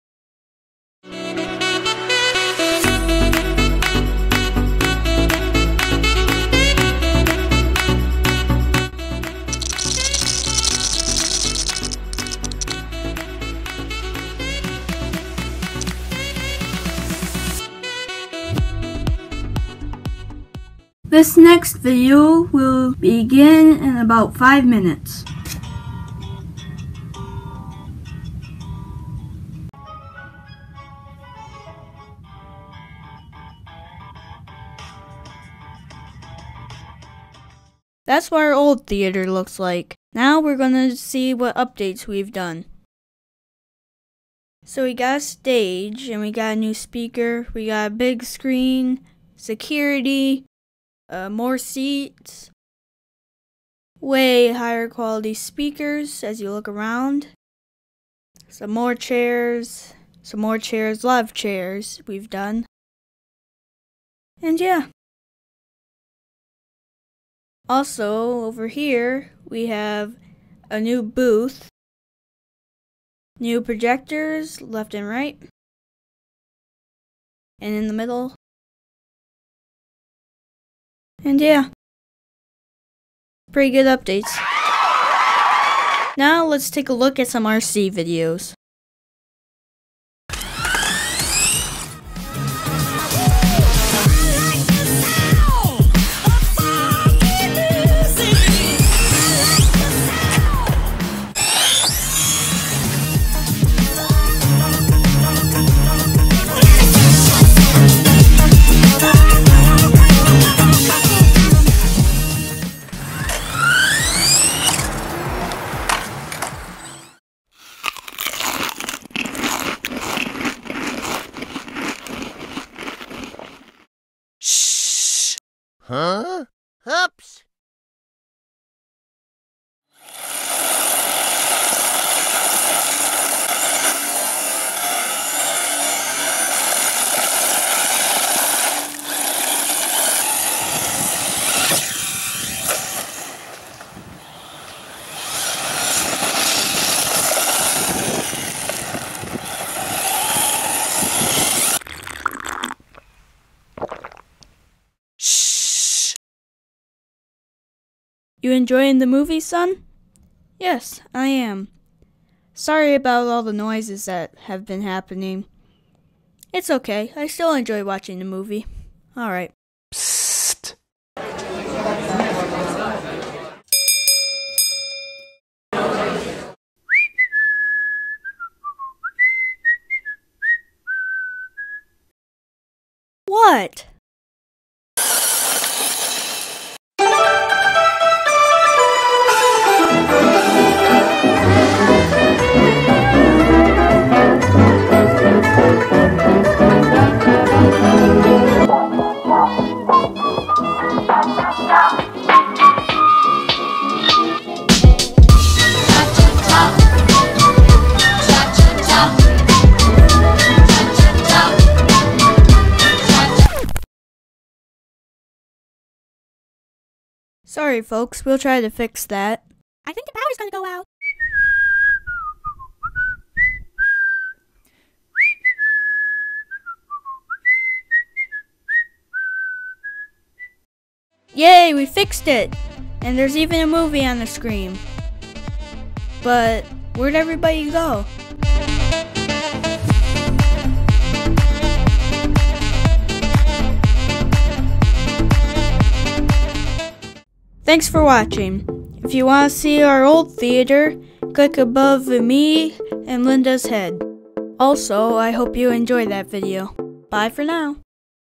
Lego Theater 2 - Fail
I re-did my Lego Theater video, this time I have cooler sets, and cooler everything!

Today we will go to the Lego Movie Theater and watch movies. But don't be to loud, otherwise other people can't hear the movie.

Rc videos by: Driverc